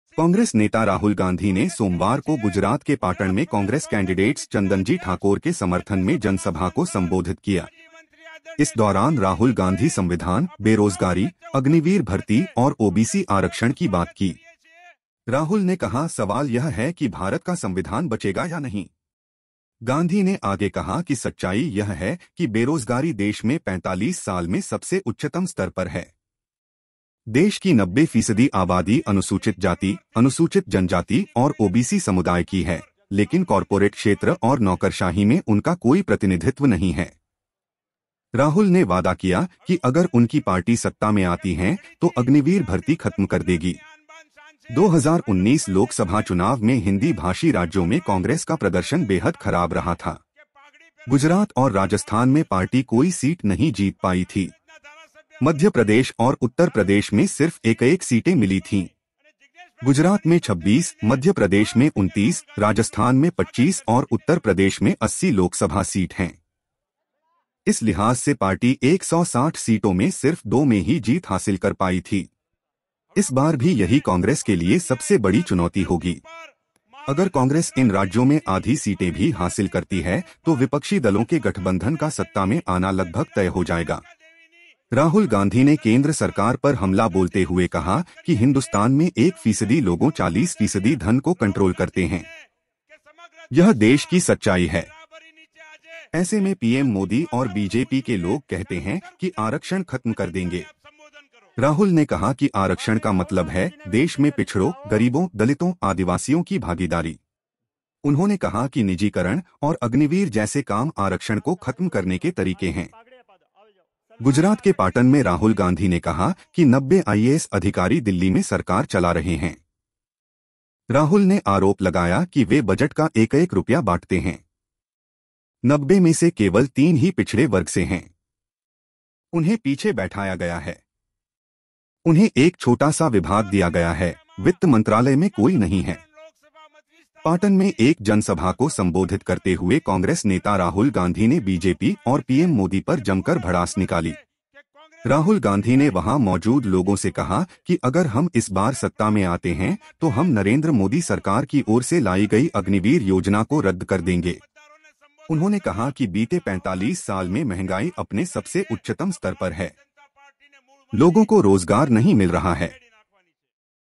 🔴गुजरात के पाटन से राहुल गाँधी ने विशाल जनसभा को संबोधित किया | Chandanji Thakor | Loksabha Election
Congress leader Rahul Gandhi Holds a MASSIVE Public Rally in Patan, Gujarat | Loksabha Elections 2024 | Chandanji Thakor | Jagdish Thakor | Shakti Sinh Gohil | Mukul Wasnik | Bharat Singh Dabhi | Jignesh Mevani | Dr. Kirit C. Patel

कांग्रेस नेता राहुल गांधी आज गुजरात के पाटन और छत्तीसगढ़ के बिलासपुर में रैली करेंगे। पाटन में राहुल कांग्रेस प्रत्याशी चंदनजी ठाकुर के समर्थन में एक विशाल जनसभा को संबोधित कर रहे हैं